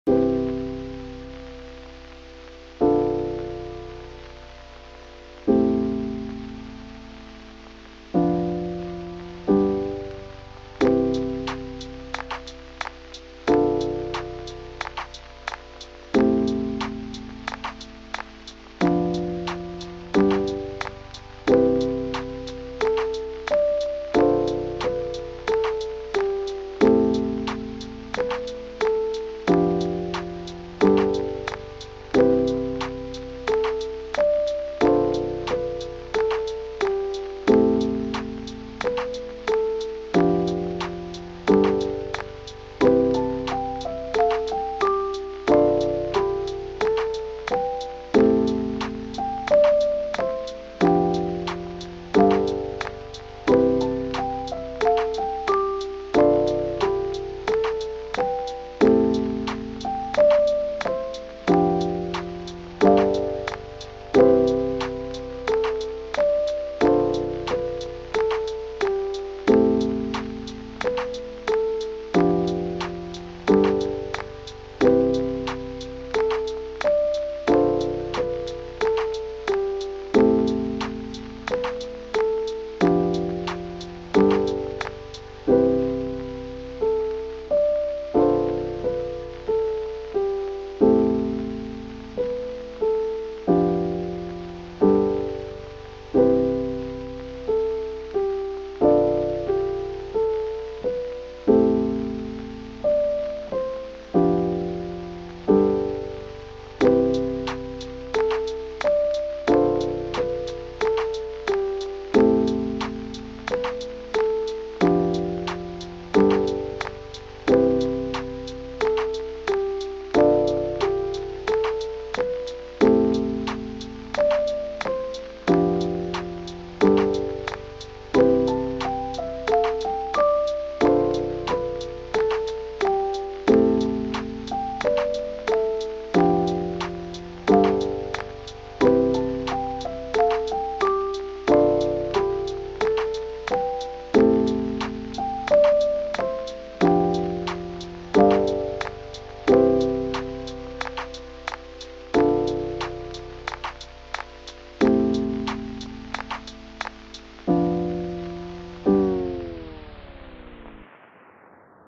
Warm Mornings | Chill Lofi
This is my first attempt at making lofi and I enjoyed every second of it! It didn't turn out too bad but I have nicer ideas for others in the near future :D